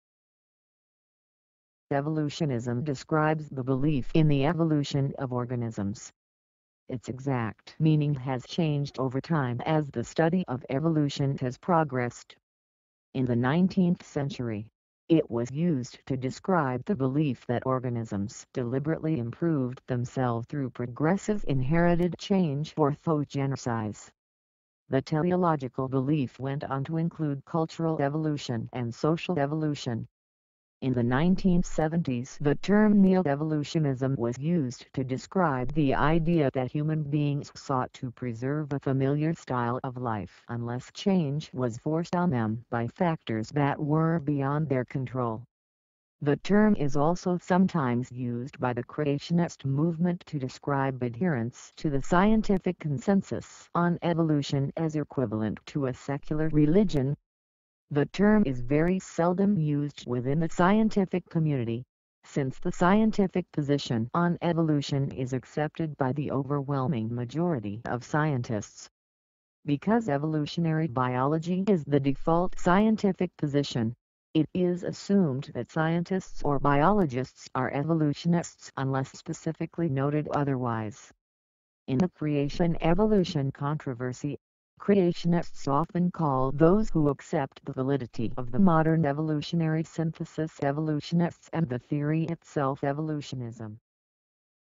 What Is EVOLUTIONISM? EVOLUTIONISM Definition & Meaning
What is EVOLUTIONISM, What does EVOLUTIONISM mean, EVOLUTIONISM meaning, EVOLUTIONISM definition, EVOLUTIONISM explanation

Evolutionism describes the belief in the evolution of organisms. Its exact meaning has changed over time as the study of evolution has progressed. In the 19th-century, it was used to describe the belief that organisms deliberately improved themselves through progressive inherited change (orthogenesis).[1][2] The teleological belief went on to include cultural evolution and social evolution.[1] In the 1970s the term Neo-Evolutionism was used to describe the idea "that human beings sought to preserve a familiar style of life unless change was forced on them by factors that were beyond their control".[3]

The term is also sometimes used by the creationist movement to describe adherence to the scientific consensus on evolution as equivalent to a secular religion.[4][5] The term is very seldom used within the scientific community, since the scientific position on evolution is accepted by the overwhelming majority of scientists.[6] Because evolutionary biology is the default scientific position, it is assumed that "scientists" or "biologists" are "evolutionists" unless specifically noted otherwise.[7] In the creation–evolution controversy, creationists often call those who accept the validity of the modern evolutionary synthesis "evolutionists" and the theory itself "evolutionism".
Source: Wikipedia.org